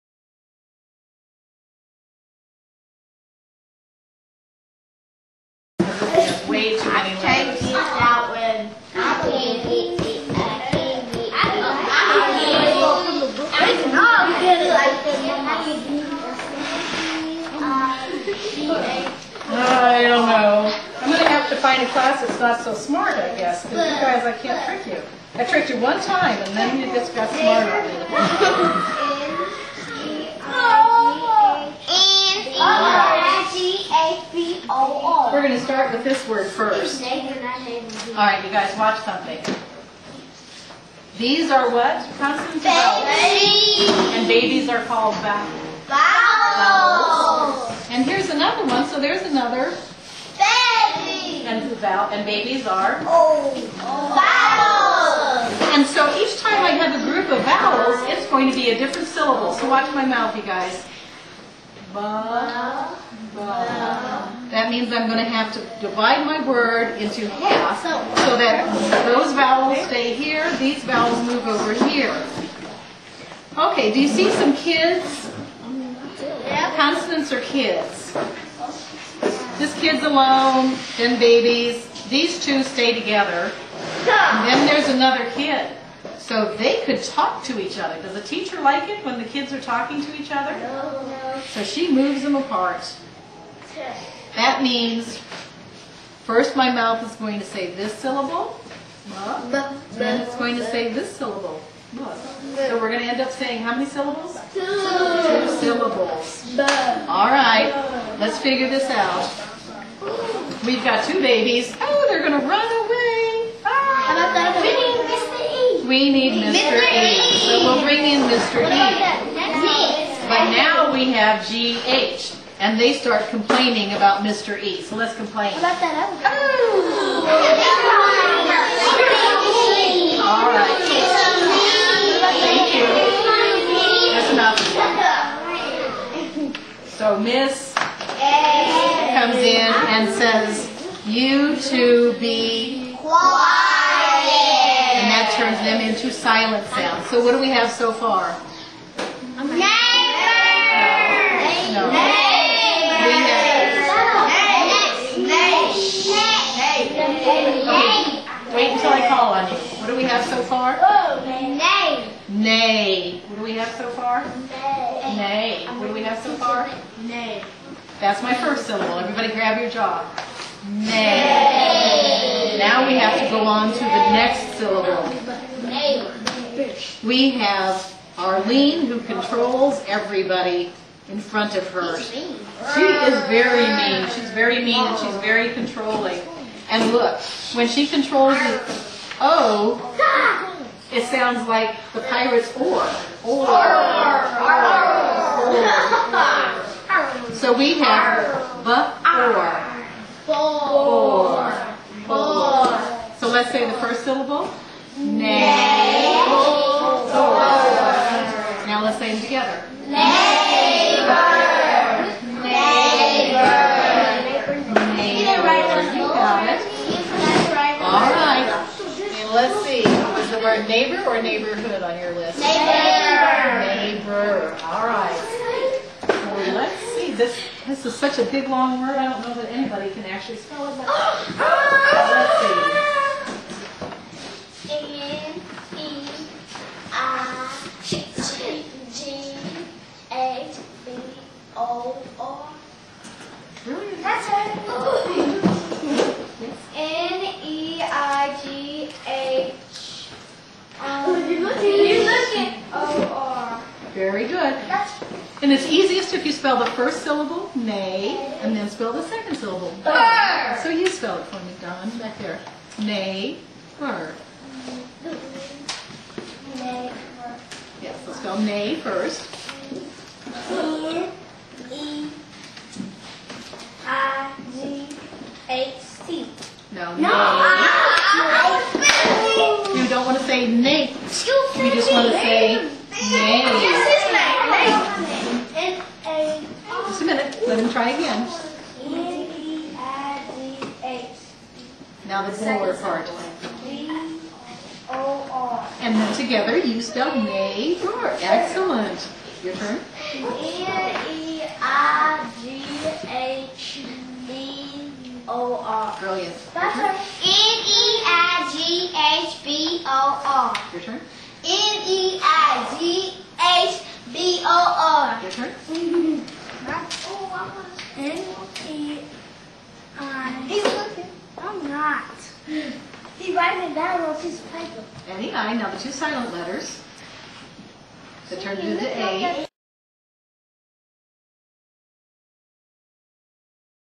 Decoding Words Using Phonic Faces
it is easy to show children principles such as breaking words between syllables, double vowels, silent letters, and r-colored vowels using the visuals provided by Phonic Faces.  Materials are available at elementOry.com